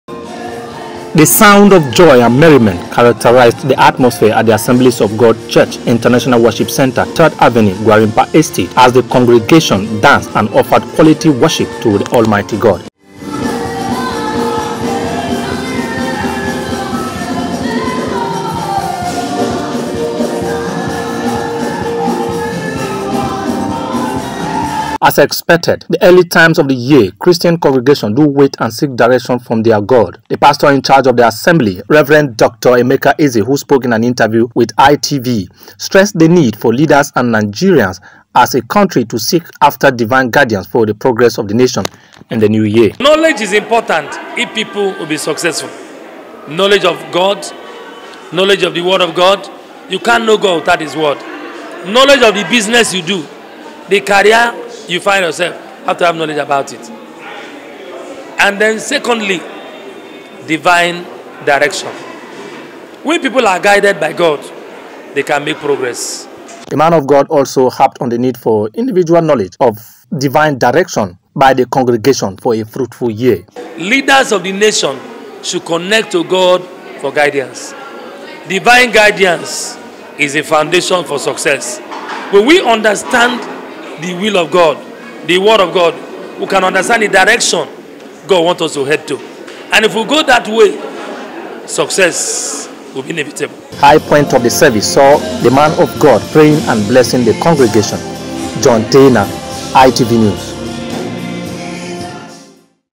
2024: Rev. Emeka Preaches Divine Guidance At Assembly Of God Church Gwarimpa
The Pastor-In-Charge Of Assemblies Of God Church International Worship Centre, 3rd Avenue, Gwarimpa, Reverend Doctor Emeka Eze, Has Emphasized The Need For Divine Guidance And Knowledge For The Leaders And People To Make Progress In The New Year 2024.

The Cleric Stated This At The Church's First Sunday Service Of The Year In Abuja.